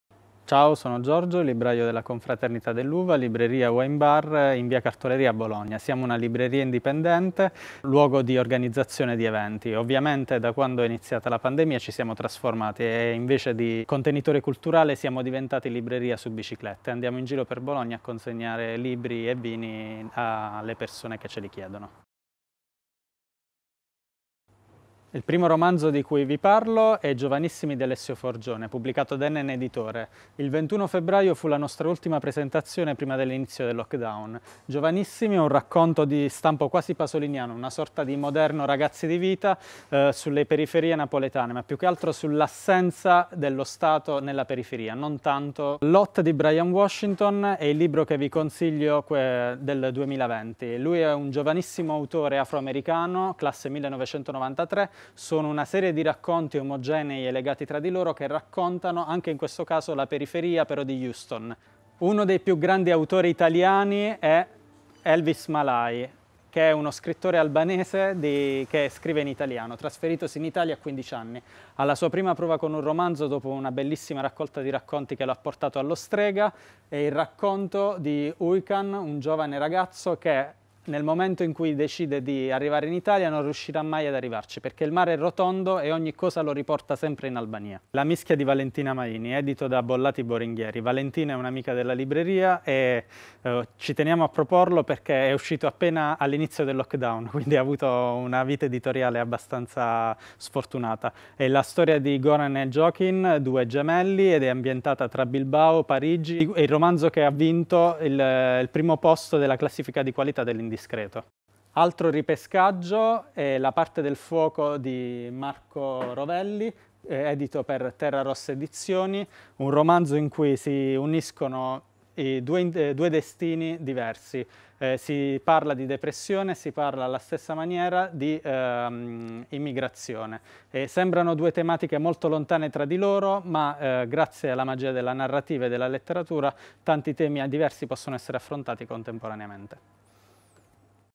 FlashChristmas2020 LETTURE: "Il libraio consiglia"-Giorgio Santangelo, per La confraternita dell'uva
I consigli sui libri del 2020 offerti da Giorgio Santangelo (La confraternita dell'uva 20b, Via Cartolerie, Bologna) per FlashChristmas 2020: 5 libri, 5 autori, 5 frasi FLASH per descriverli!

Network dell'Informagiovani Multitasking del Comune di Bologna
U.I.Servizi per i giovani.